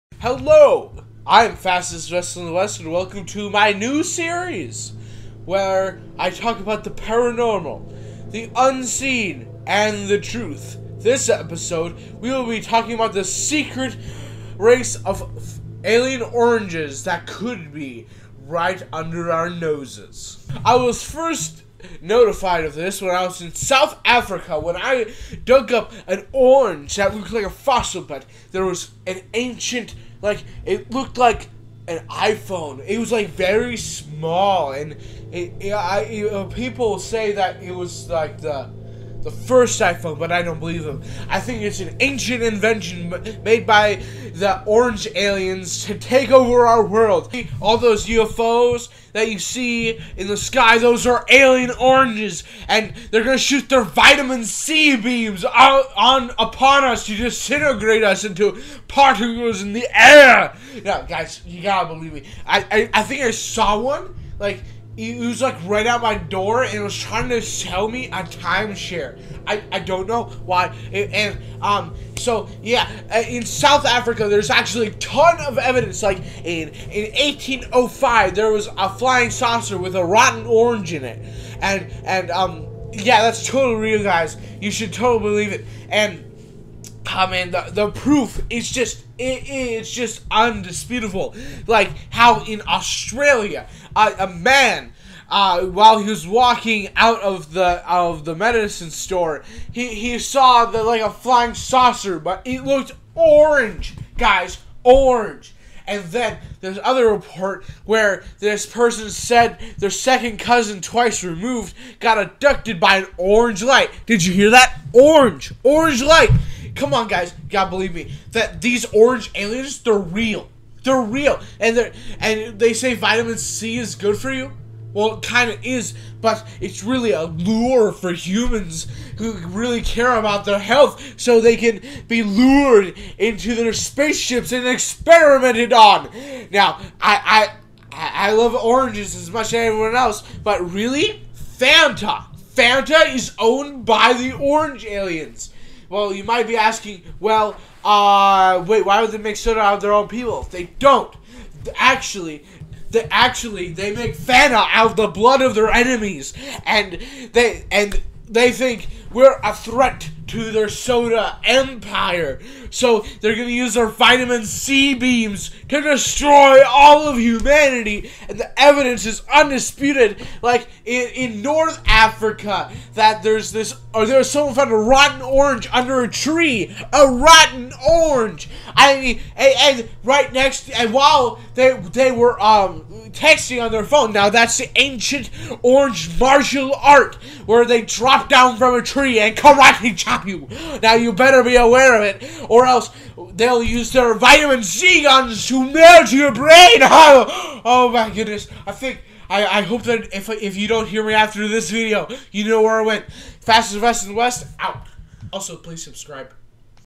Explained Mysteries EP1
WARNING: THIS IS A JOKE DO NOT TAKE THE SERIOUSLY.

Welcome to my new series Explained Mystery's were we talk about all kinds of mystery's, and find the TRUTH.

This episode: A secret race of alien oranges that want to wipe out the human race.